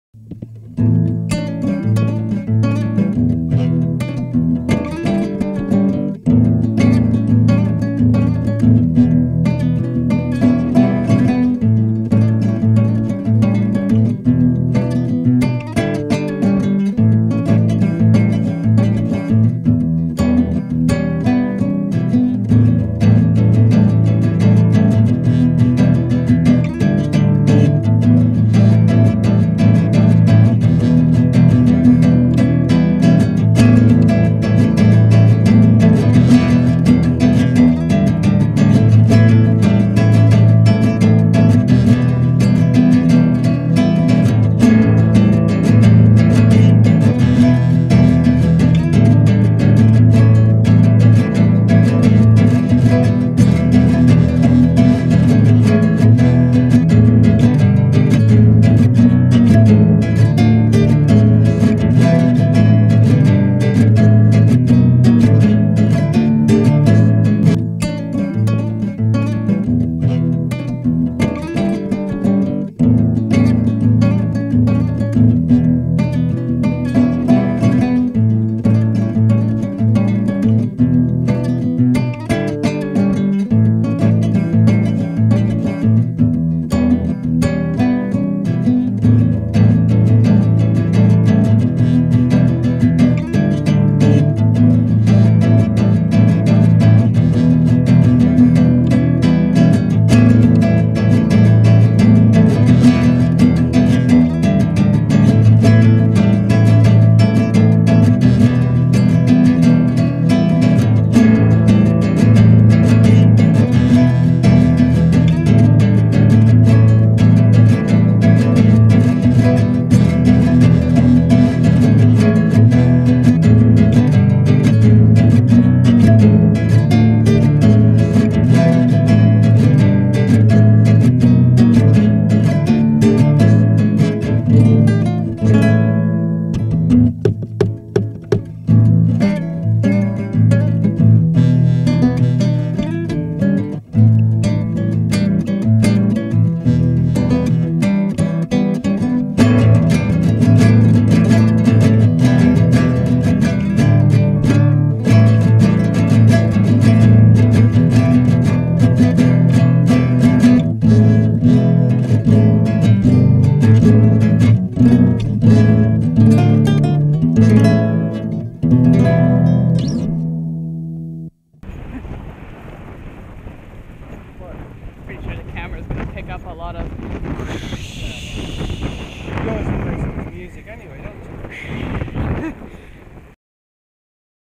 Biking Journeys: Tentsmuir Forest
J and I went for spin through the "working" forest called Tentsmuir National Nature Reserve near Dundee! It was a good thing we had off-road bikes because we were dodging stones and skidding through sand. It was a blast!